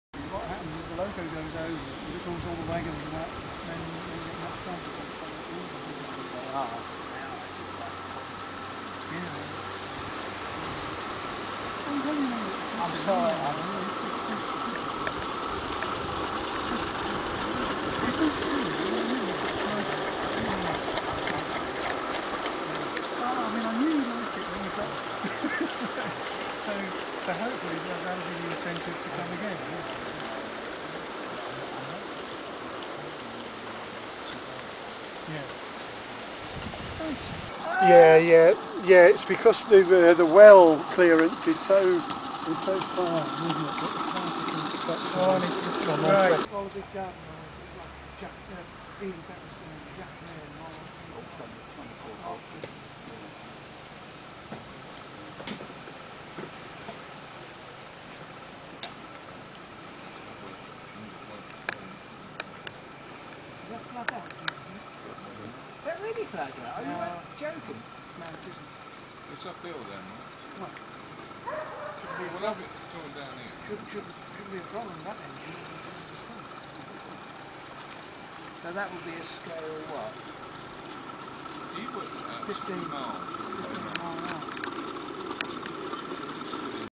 7F 0-8-0 in my garden.wmv
Visiting train plodding around the garden, as the rain starts to spot.